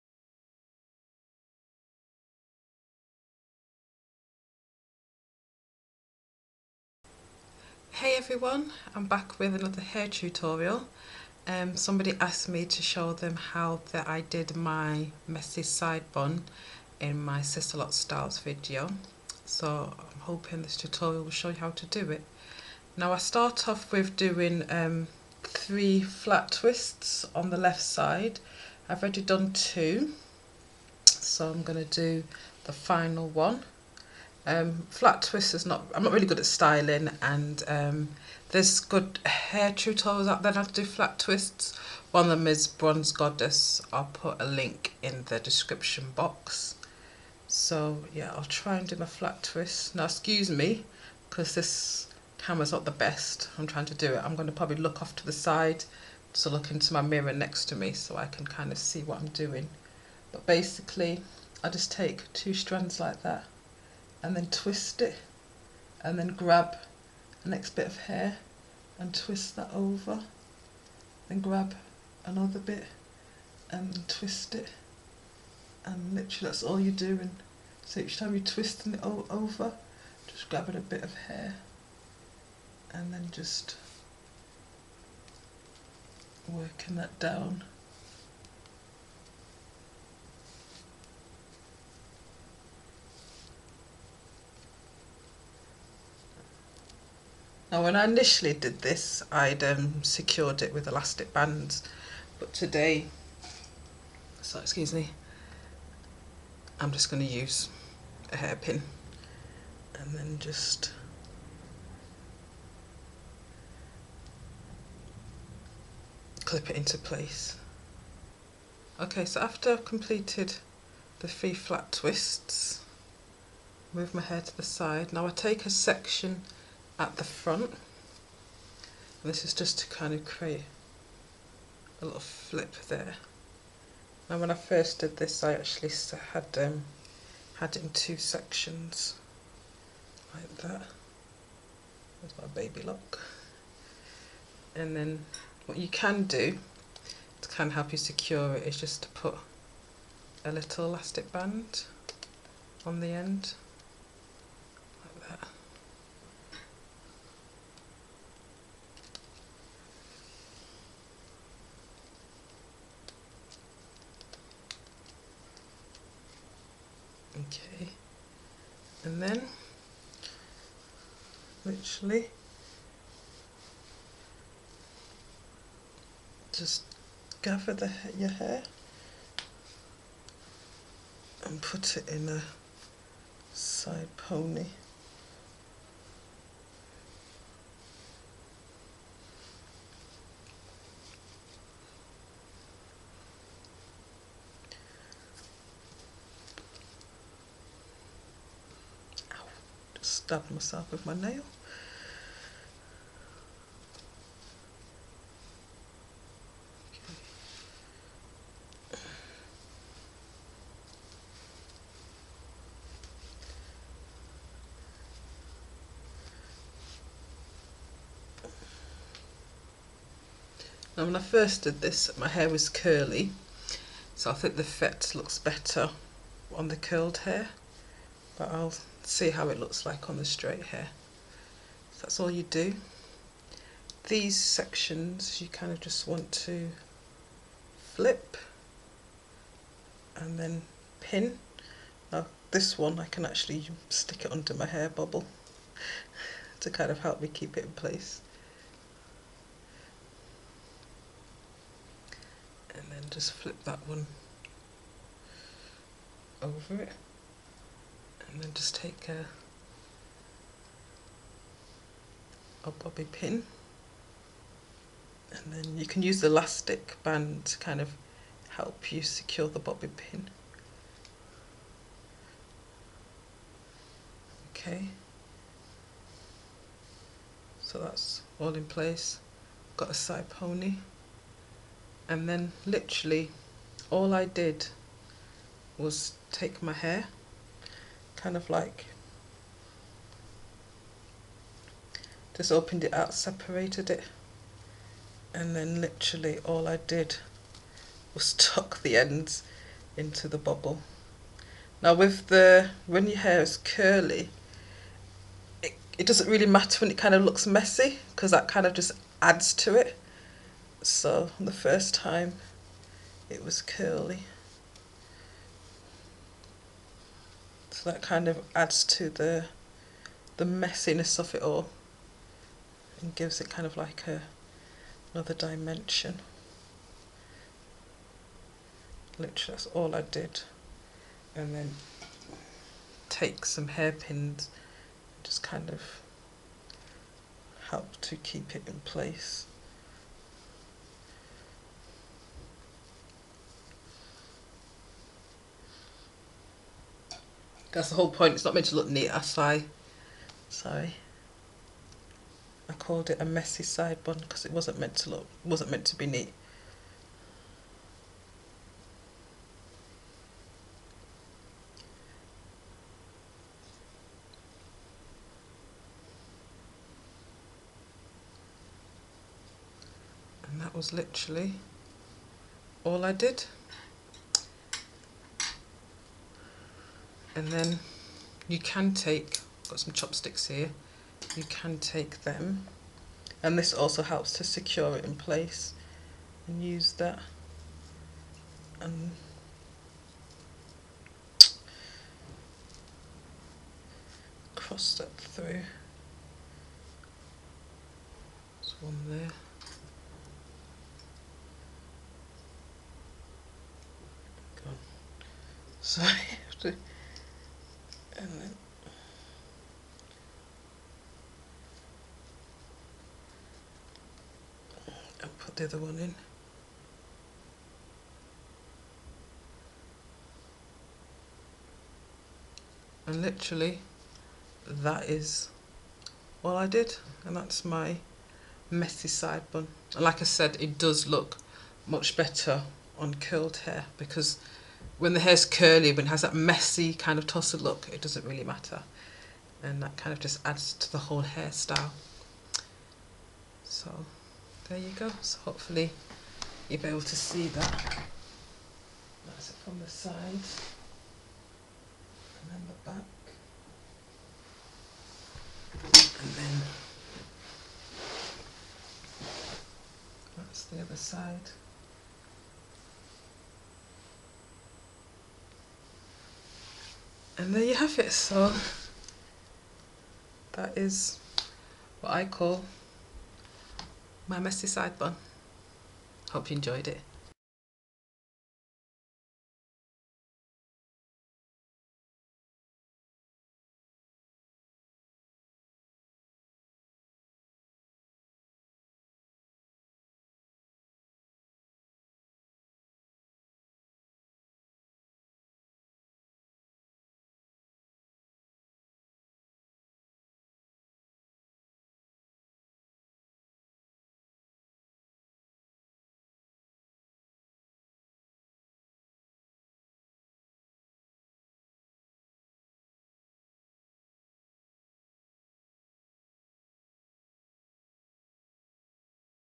Sisterlocks Hair Tutorial: Messy Side Bun
As requested a hair tutorial of how i did my messy side bun in my sisterlocks style video